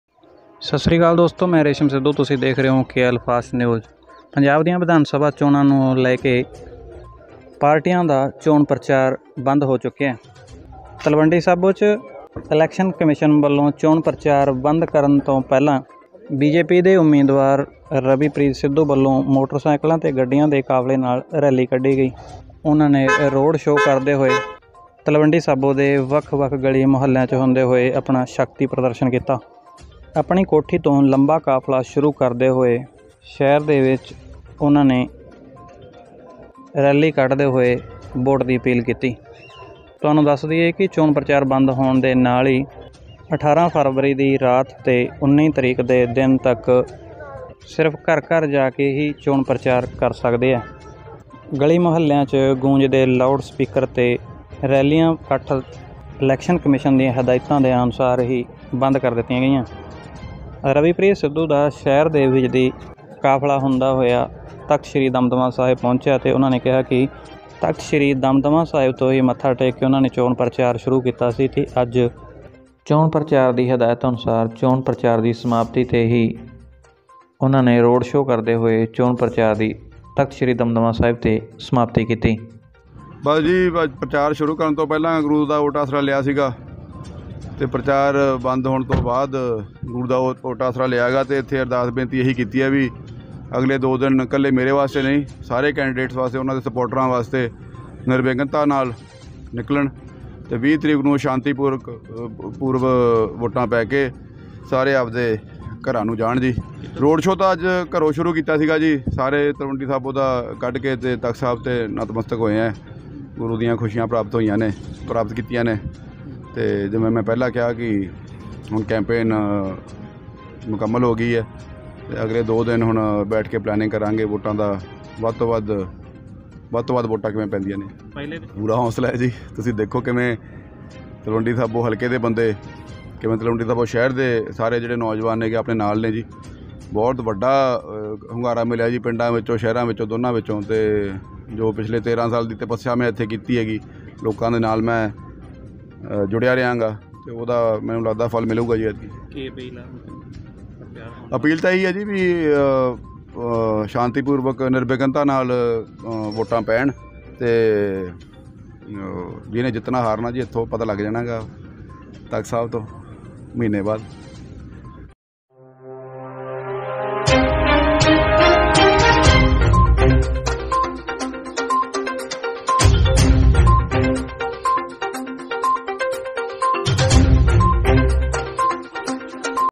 ਮੋਟਰਸਾਈਕਲਾਂ ਤੇ ਕੱਢਿਆ ਰੋਡ ਸ਼ੋ, ਤਖ਼ਤ ਸ੍ਰੀ ਦਮਦਮਾ ਸਾਹਿਬ ਸਮਾਪਤੀ ਕਰ ਰਵੀਪ੍ਰੀਤ ਸਿੱਧੂ ਹੋਏ ਨਤਮਸਤਕ,
ਚੋਣ ਪ੍ਰਚਾਰ ਬੰਦ ਹੋਣ ਤੋਂ ਪਹਿਲਾਂ ਕੱਢਿਆ ਰੋਡ ਸ਼ੋ, ਤਲਵੰਡੀ ਸਾਬੋ,